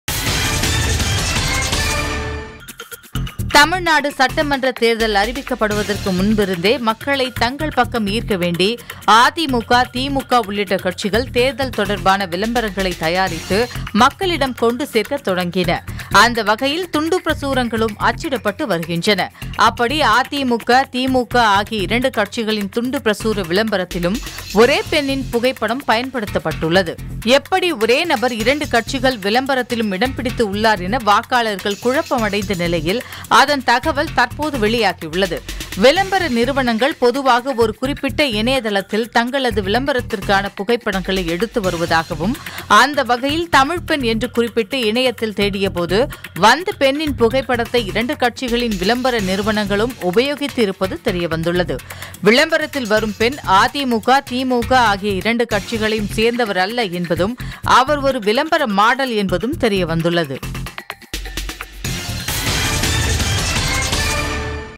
ஒரே மாடலை விளம்பரத்திற்கு உபயோகித்த அதிமுக, திமுக | செய்தித் துளிகள்
ஒரே மாடலை விளம்பரத்திற்கு உபயோகித்த அதிமுக, திமுக | செய்தித் துளிகள்| செய்தித் துளிகள்